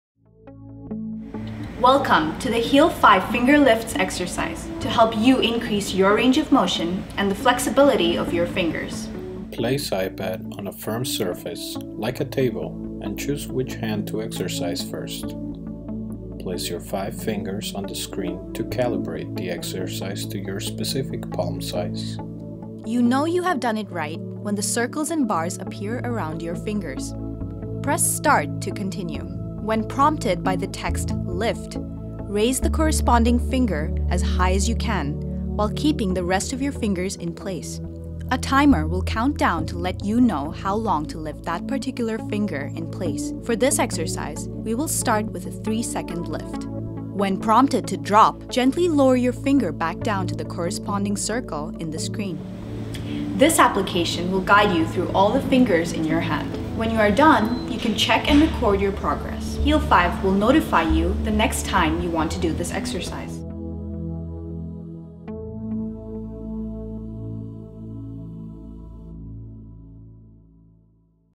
Heal 5: Instructional Video
Heal 5: For people suffering from arthritis pain who want a different way to alleviate their pain than medication. The Heal 5 iOS application is an interactive exercise routine using touchscreen and multitouch technology that teaches users how to exercise, gives them a platform to make goals and receive notifications to keep them on track. Unlike other arthritis applications the Heal 5 focuses on exercise as a way to help pain rather than tracking medication.